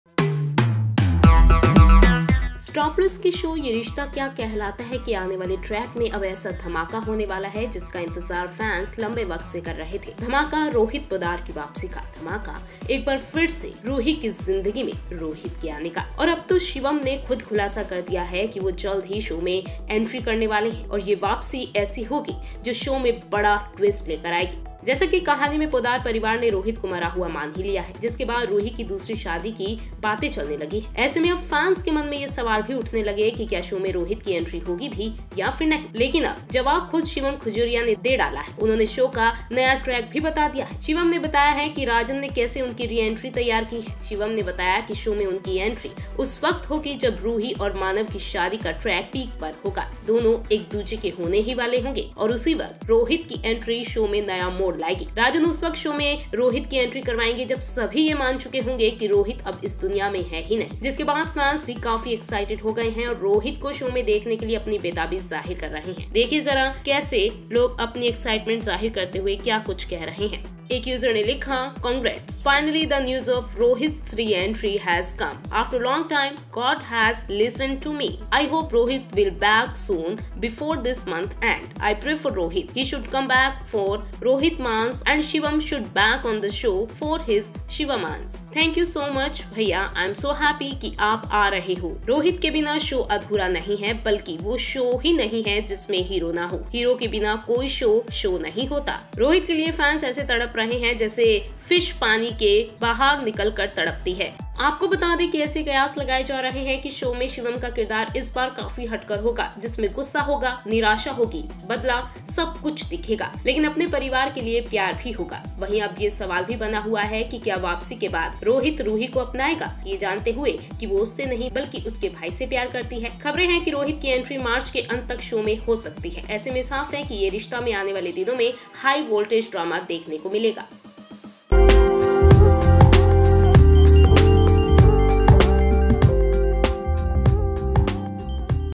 YRKKH This is How Rohit Will Return in Track Fans Showing Their Excitement After Shivam Revealution
Rohit aka Shivam Khajuria To Return Soon in The Track ! Check Out The Video !